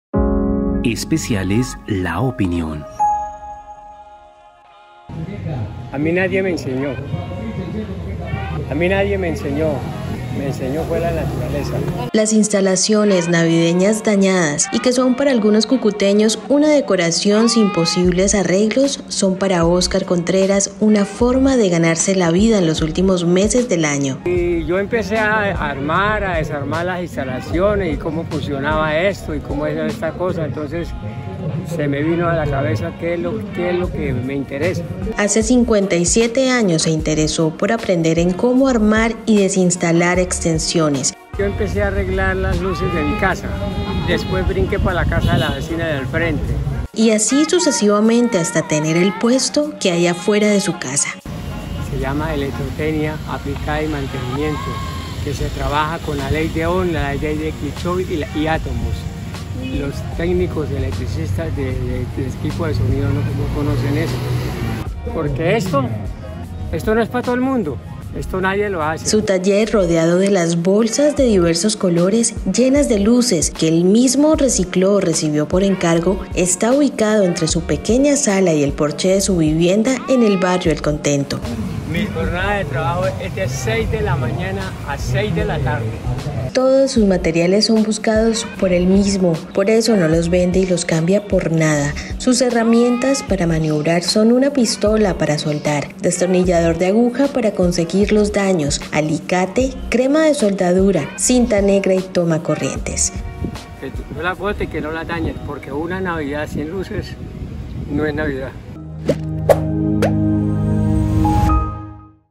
Especiales La Ó | Conozca la historia del cucuteño que revive las luces navideñas
Las instalaciones navideñas dañadas, y que son para algunos cucuteños una decoración sin posibles arreglos, son para Óscar Contreras una forma de ganarse la vida en los últimos meses del año.

Hace 57 años se interesó por aprender el cómo armar y desinstalar extensiones, así que de forma empírica inició por las de su hogar, luego por las de sus vecinos y así sucesivamente hasta tener el puesto que hay afuera de su casa.